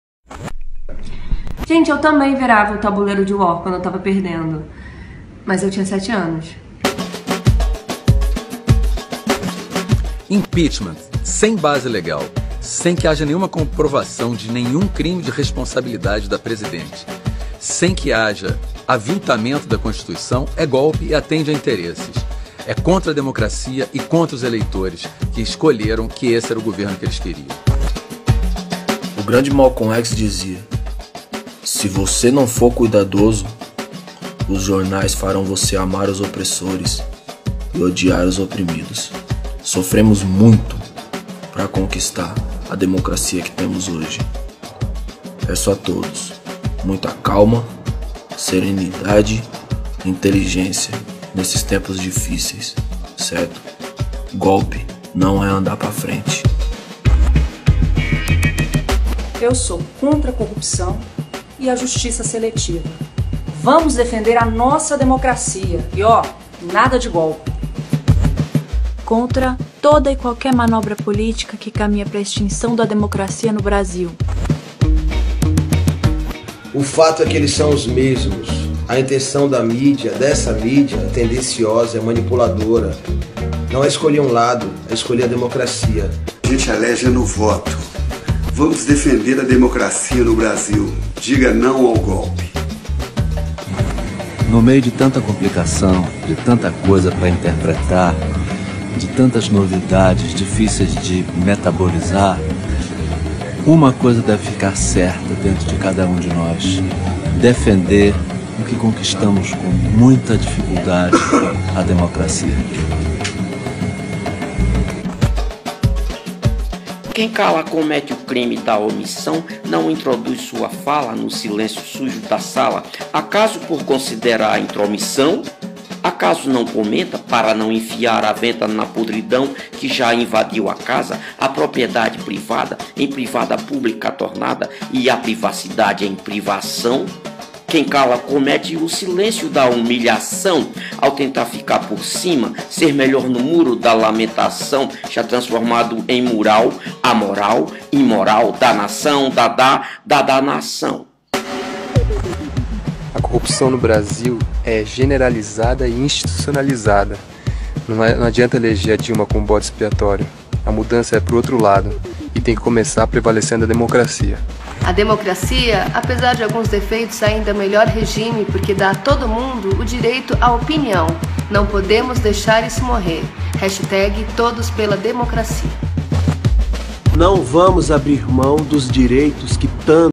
GOLPE NUNCA MAIS!
31 de março de 2016

Caetano Veloso, Emicida, Clarice Falcão, Leoni e vários outros artistas explicam porque o impeachment é golpe e também a importância da democracia.

‪#‎VemPraDemocracia‬ ‪#‎BrasilContraOGolpe‬
‪#‎EquipeMR‬